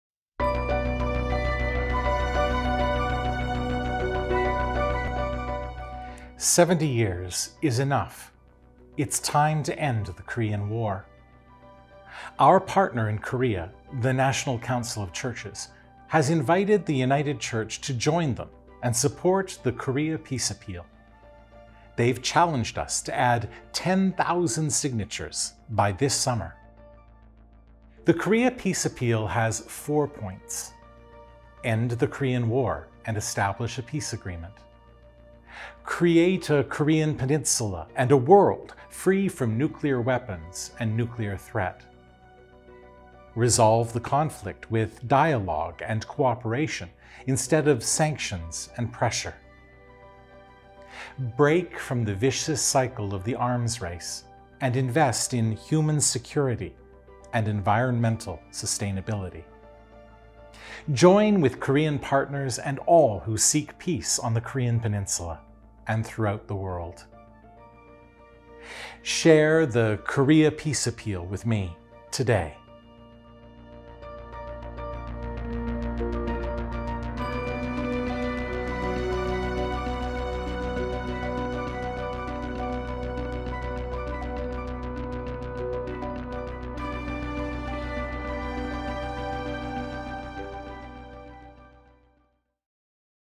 End the Korean War
The United Church of Canada Moderator urges endorsement of the Korea Peace Appeal 10,000 Signature Campaign and highlights the four elements of the appeal.